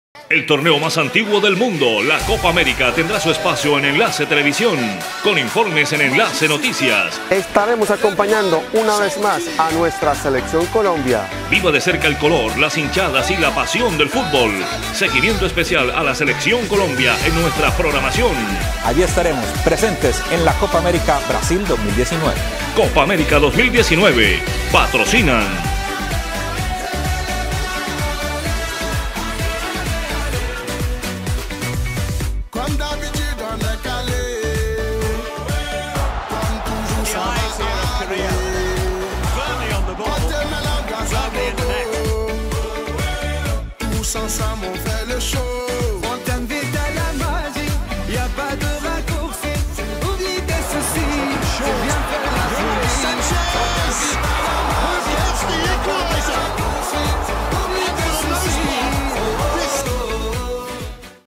Pasión por el deporte presente en Copa América Brasil .
Gracias a nuestros patrocinadores por la confianza y credibilidad lo que nos permite estar una vez más en la Copa América y de regreso a tierra del jogo bonito. Pasión por el deporte y 5 en Deportes dicen presente en Brasil del 14 de junio al 07 de julio, informes en pasión por el deporte radio a través de Yariguies Stereo, Enlace Noticias y Especiales copa América los días lunes y jueves a las 9:30 pm.